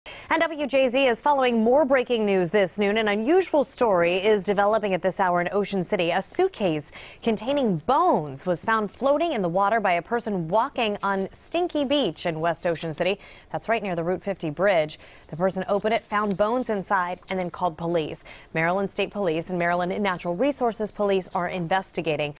Bones Found In Suitcase On Ocean City Beach
Multiple law enforcement agencies are in Ocean City to help investigate after bones were found in a suitcase.